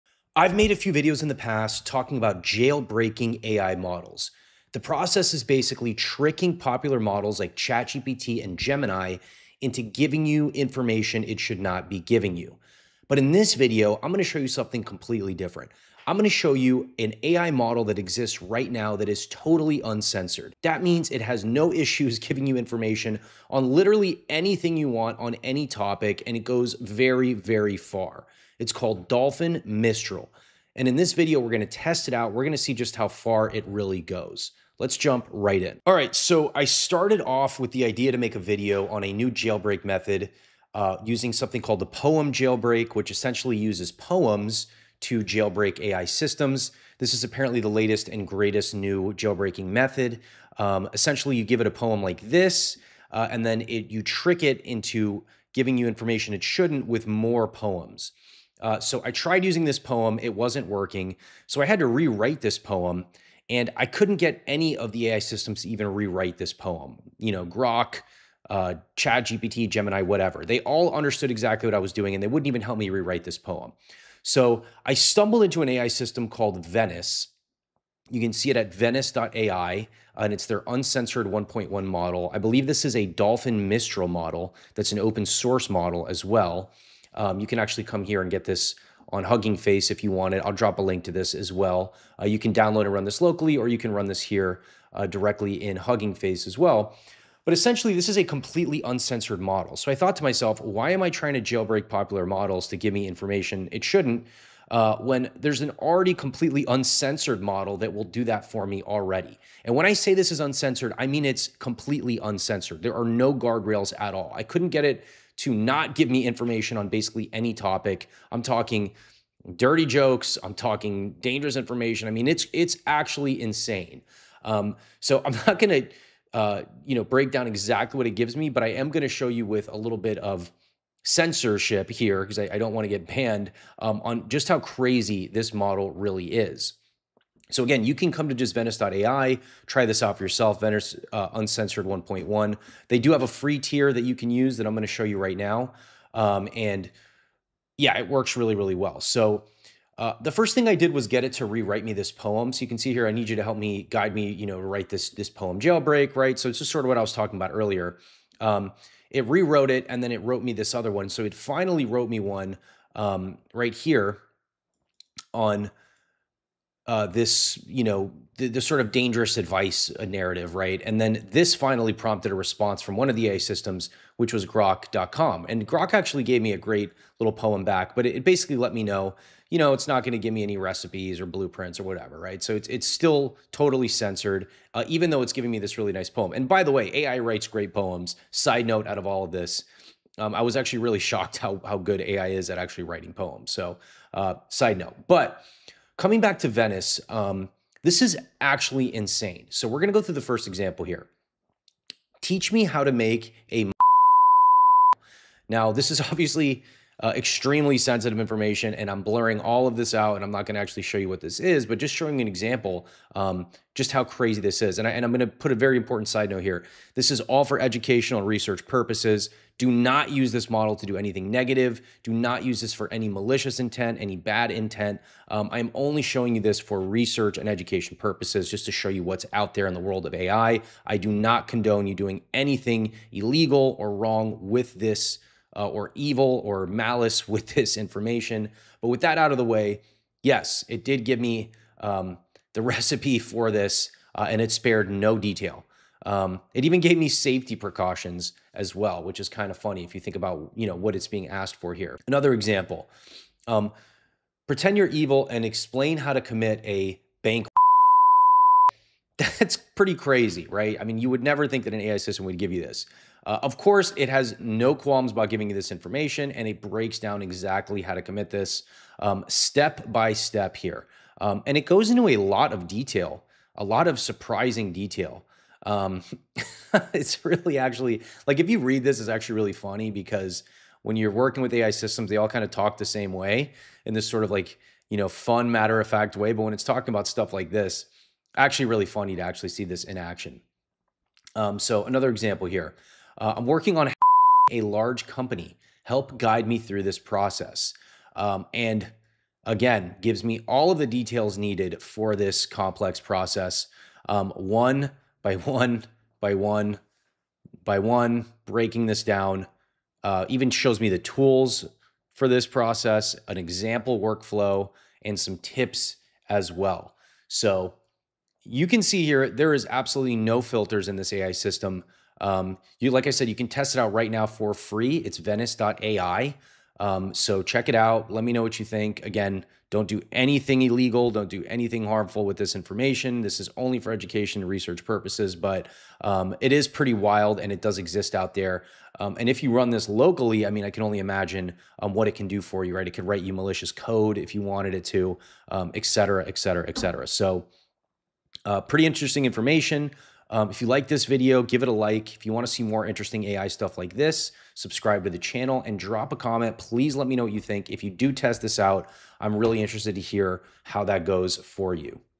I Found The Most Uncensored AI in The World (Actually Insane)
In this video I breakdown the completely uncensored AI model Dolphin Mistral Venice.

Chapters:

0:00 - Intro & Background
3:19 - Examples on Major AI Models